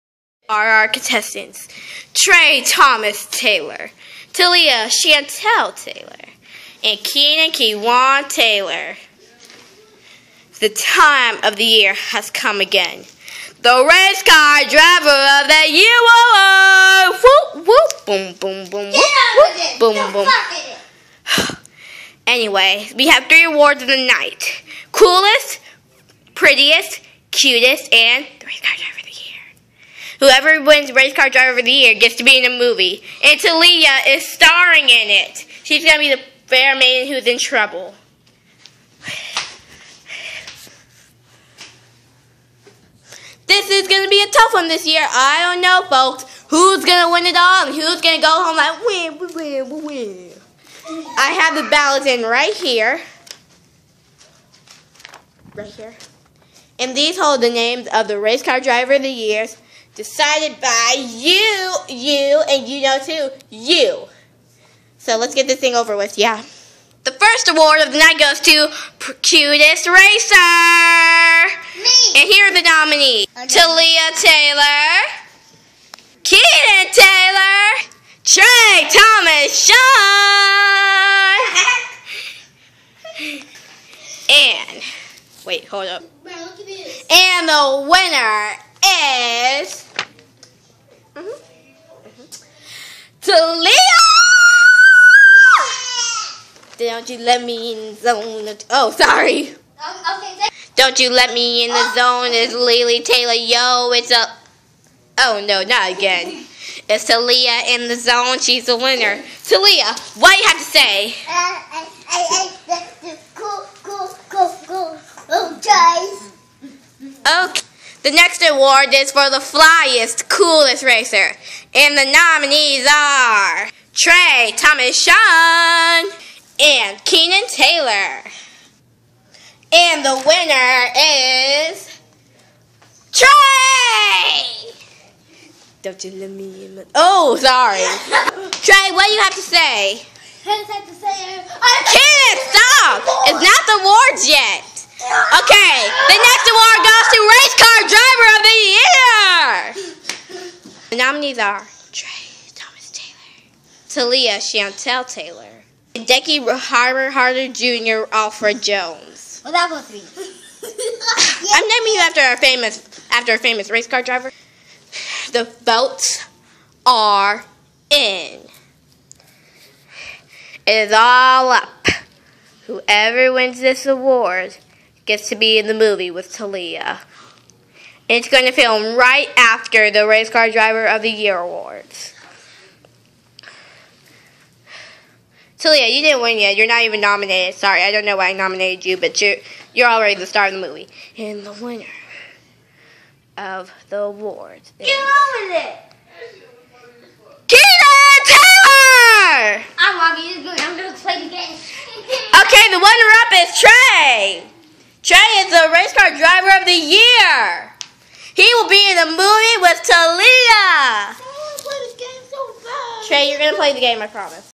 The race car driver of the year awards
The day has come the racers are ready for this! I know that you love our crazy racers so we made an award show.